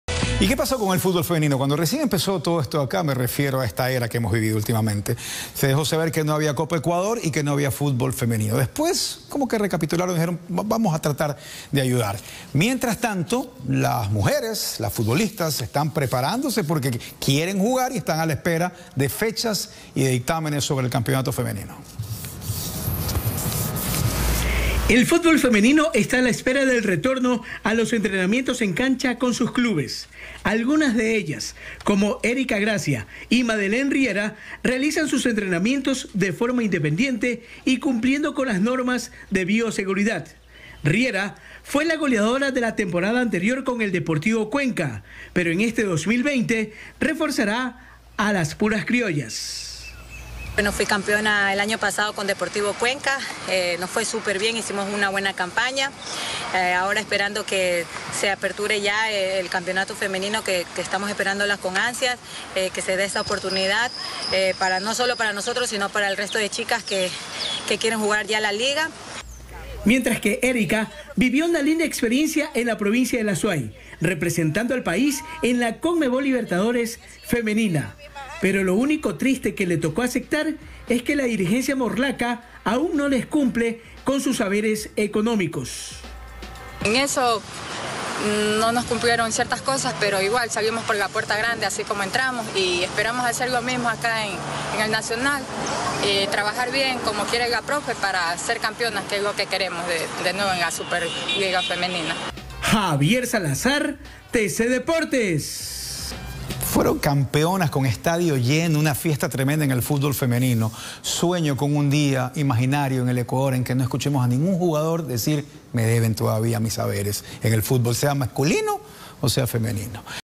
D.Cuenca femenino fueron campeonas y siguen sin cobrar su dinero
Dirigencia azuaya disfrutó del éxito fútbolistico y no fueron consecuentes con sus jugadoras, ahora solo esperan la voluntad de ellas.

Cortesía Tc Deportes